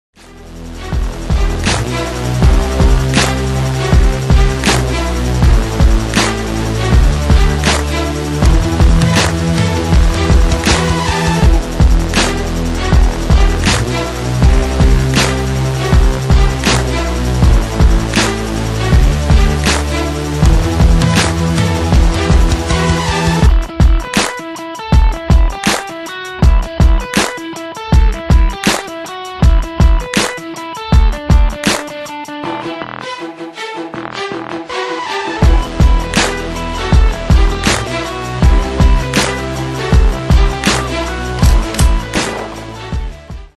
Super Ultimate Snapchange
Here's me doing the snapchange but a harder version :D (the 2nd on the video)
I saw someone here doing this with 4 aces.. I improved it up to 8 aces :)